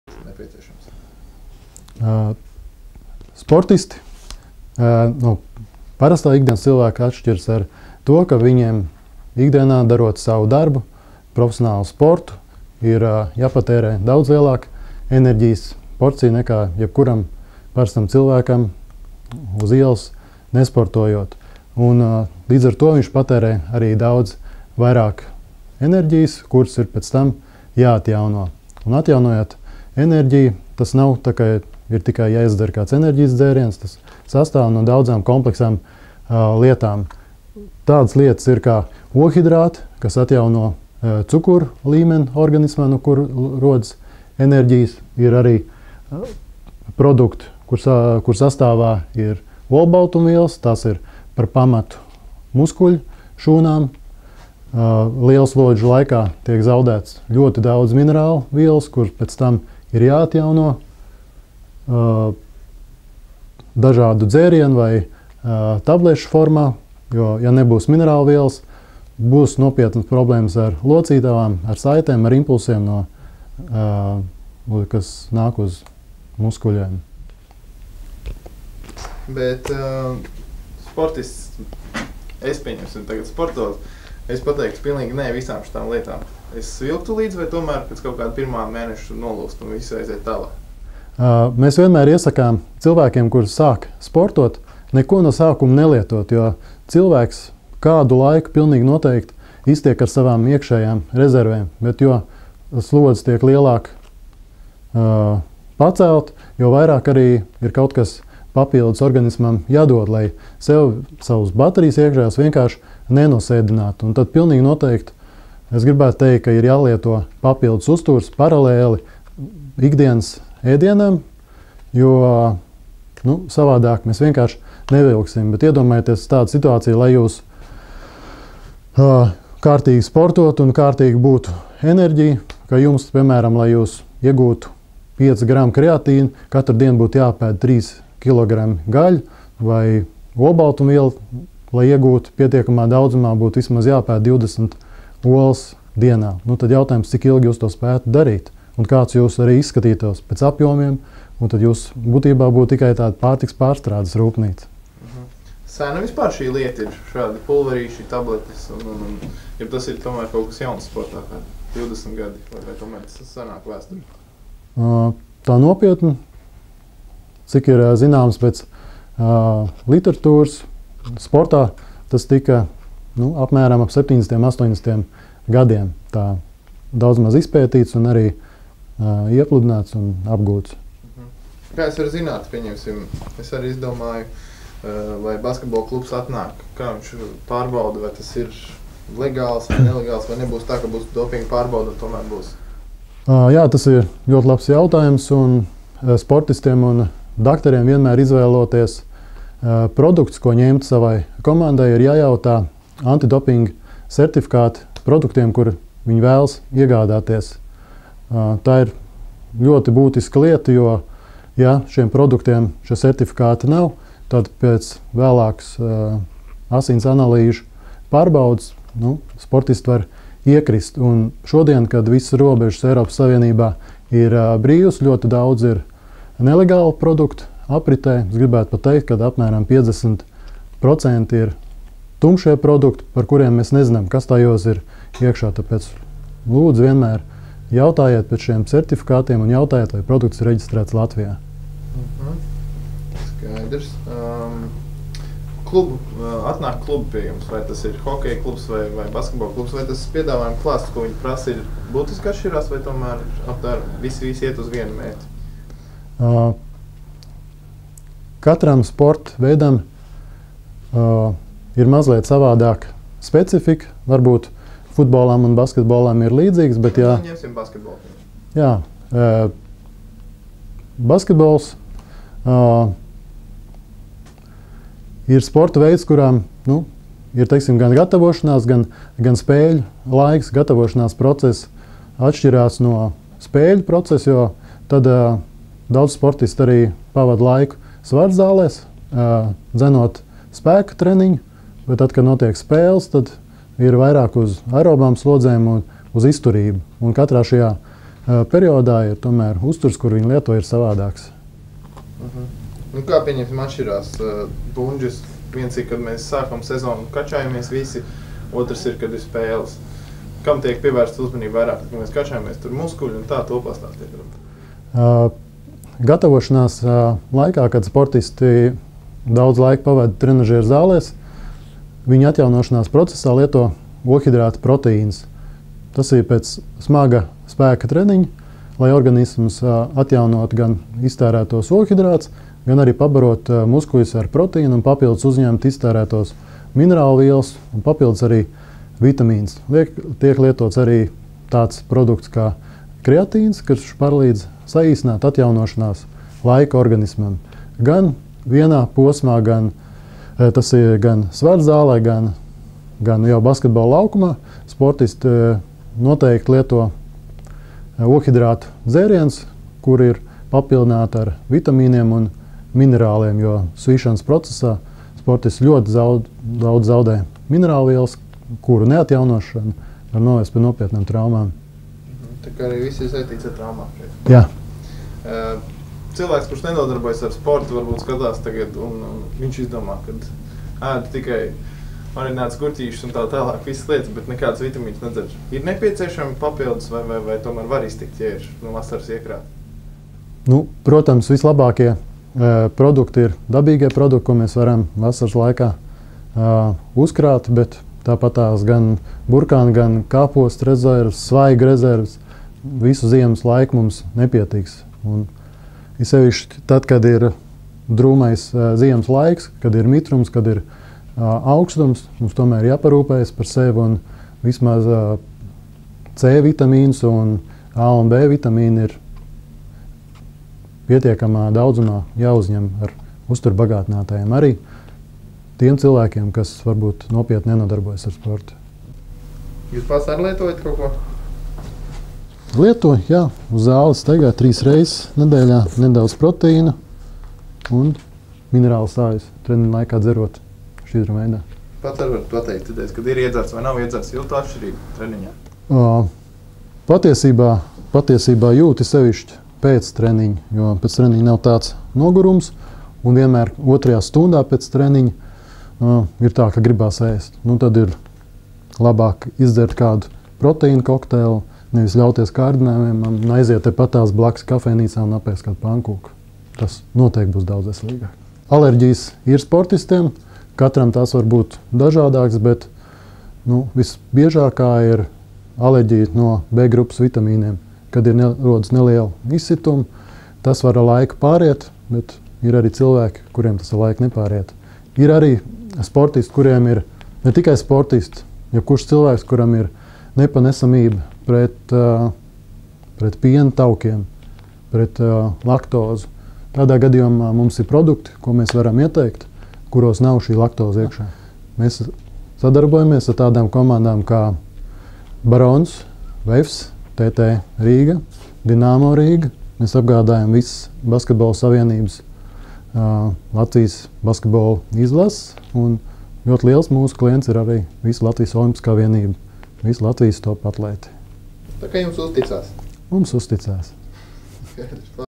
NAGLIS VITAMINI.avi
Māris Naglis par sportošanu, vitamīniem un uztura bagātinātājiem.